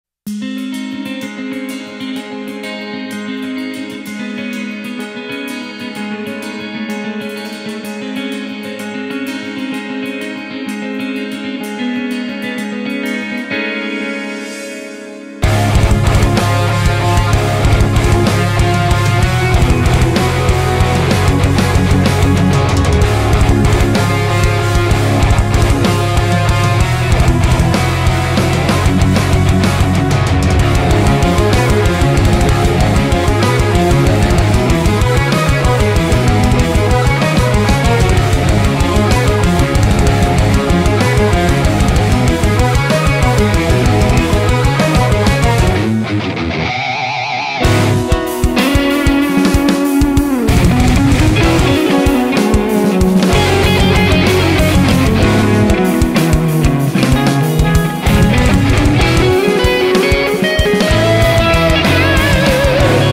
Testing out a HUFSCHMID Alien HUFGLOW Pick
Few riffs with one of the new Hufschmid guitar picks. For the size/thickness of these picks they’re surprisingly really easy to play